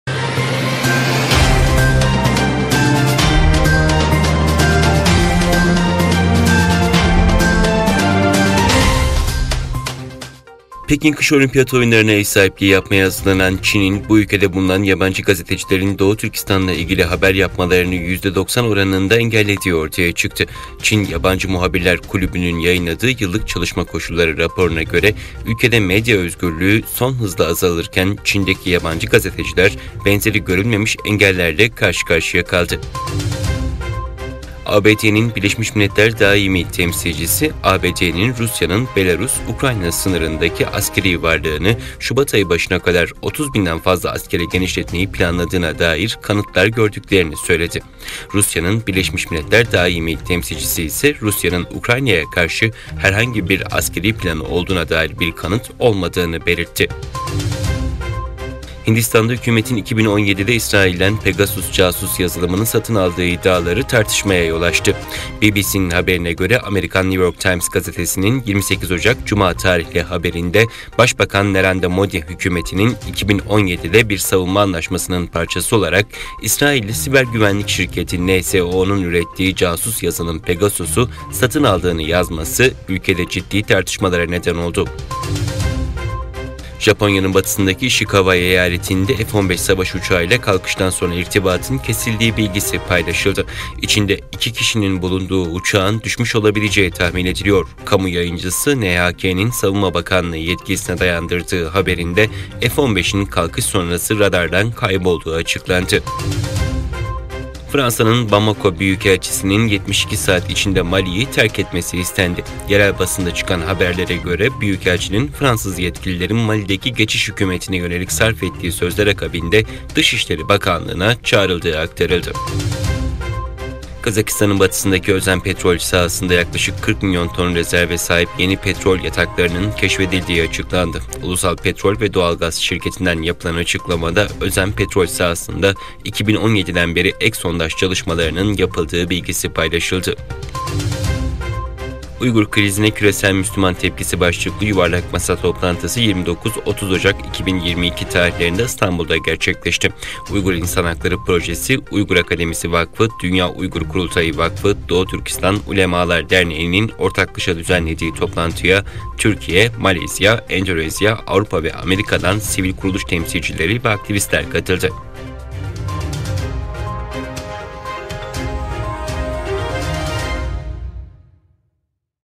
01/02/2022 Çin, yabancı gazetecilerin Doğu Türkistan haberi yapmasını 'yüzde 90' engelledi
Çin, yabancı gazetecilerin Doğu Türkistan haberi yapmasını 'yüzde 90' engelledi
Pekin Kış Olimpiyat Oyunları'na ev sahipliği yapmaya hazırlanan Çin'in, bu ülkede bulunan yabancı gazetecilerin Doğu Türkistan'la ilgili haber yapmalarını yüzde 90 oranında engellediği ortaya çıktı. Çin Yabancı Muhabirler Kulübü'nün (FCCC) yayınladığı yıllık 'çalışma koşulları' raporuna göre, ülkede medya özgürlüğü "son hızla" azalırken Çin'deki yabancı gazeteciler "benzeri görülmemiş engellerle" karşı karşıya kaldı.

ABD: Rusya, Ukrayna sınırındaki asker sayısını artırıyor
ABD'nin BM Daimi Temsilcisi Linda Thomas-Greenfield, ABD'nin Rusya'nın Belarus-Ukrayna sınırındaki askeri varlığını şubat ayı başına kadar 30 binden fazla askere genişletmeyi planladığına dair kanıtlar gördüklerini söyledi. Rusya'nın BM Daimi Temsilcisi Nebenzya ise Rusya'nın Ukrayna'ya karşı herhangi bir askeri planı olduğuna dair bir kanıt olmadığını belirtti.

Hindistan'da hükümetin Pegasus casus yazılımını satın aldığı iddiaları tartışılıyor
Hindistan'da, hükümetin 2017'de İsrail'den Pegasus casus yazılımını satın aldığı iddiaları tartışmaya yol açtı. BBC'nin haberine göre, Amerikan New York Times gazetesinin 28 Ocak Cuma tarihli haberinde, Başbakan Narendra Modi hükümetinin 2017'de bir savunma anlaşmasının parçası olarak İsrailli siber güvenlik şirketi NSO'un ürettiği casus yazılım Pegasus'u satın aldığını yazması, ülkede ciddi tartışmalara neden oldu.

Japonya'da bir F-15 kalkış sonrası radardan kayboldu
Japonya'nın batısındaki İşikawa eyaletinde F-15 savaş uçağıyla kalkıştan sonra irtibatın kesildiği bilgisi paylaşıldı. İçinde iki kişinin bulunduğu uçağın düşmüş olabileceği tahmin ediliyor. Kamu yayıncısı NHK'nin Savunma Bakanlığı yetkilisine dayandırdığı haberinde, F-15'in kalkış sonrası radardan kaybolduğu açıklandı.

Mali, Fransa Büyükelçisi'nden ülkeyi terk etmesini istedi
 Fransa'nın Bamako Büyükelçisi Joel Meyer'den 72 saat içinde Mali'yi terk etmesi istendi. Yerel basında çıkan haberlerde, Joel Meyer'in, Fransız yetkililerin Mali'deki geçiş hükümetine yönelik sarf ettiği sözler akabinde Dışişleri Bakanlığına çağrıldığı aktarıldı.

Kazakistan'da yeni petrol yatakları keşfedildi
Kazakistan'ın batısındaki Özen petrol sahasında yaklaşık 40 milyon ton rezerve sahip yeni petrol yataklarının keşfedildiği açıklandı. Ulusal Petrol ve Doğal Gaz Şirketi KAZMUNAYGAZ'dan yapılan açıklamada, Özen petrol sahasında 2017'den beri ek sondaj çalışmalarının yapıldığı bilgisi verildi.

"Uygur Krizine Küresel Müslüman Tepkisi" başlıklı toplantı gerçekleşti
"Uygur Krizine Küresel Müslüman Tepkisi" başlıklı Yuvarlak Masa Toplantısı 29-30 Ocak 2022 tarihinde İstanbul'da gerçekleşti. Uygur İnsan Hakları Projesi, Uygur Akademisi Vakfı, Dünya Uygur Kurultayı Vakfı, Doğu Türkistan Ulemalar Derneğinin ortaklaşa düzenlediği toplantıya Türkiye, Malaysiya, Endonezya, Avrupa ve Amerika’dan sivil kuruluş temsilcileri ve aktivistler katıldı.